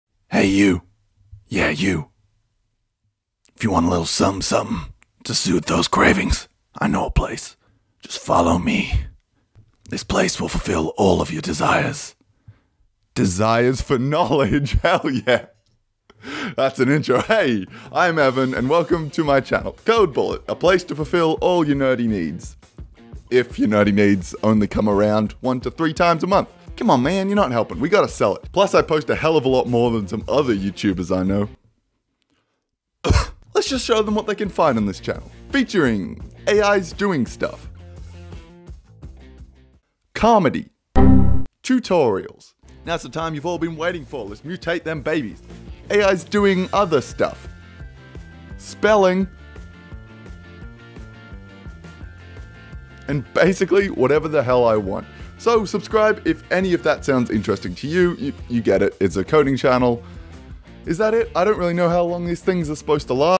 Code Bullet Channel Trailer
I was bored and wanted to have a channel trailer so here it is.